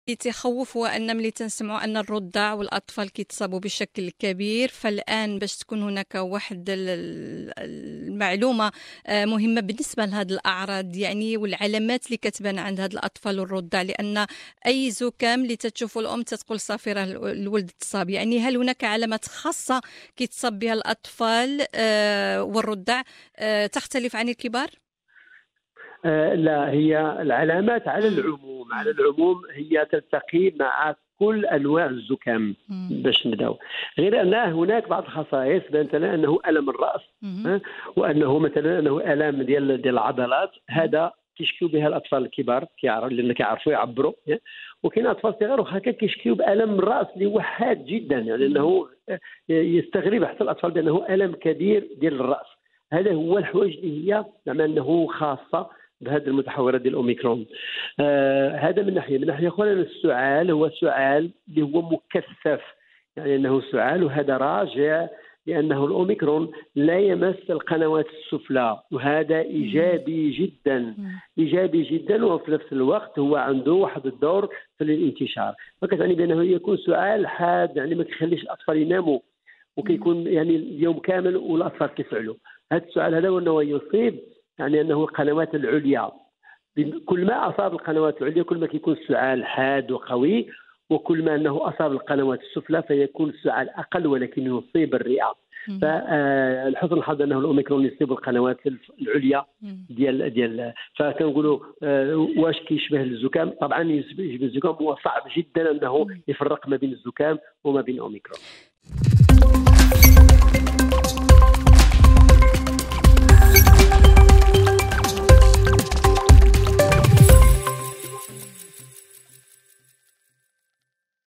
أوميكرون وتأثيره على الأطفال والرضع
متحور أوميكرون لا يستثني الأطفال والرضع ولكنه ضعيف، تفاصيل حول مدى تأثير هذا المتحور على الأطفال وصغار السن مع الدكتور دحان نور الدين ضيف صباح الأسرة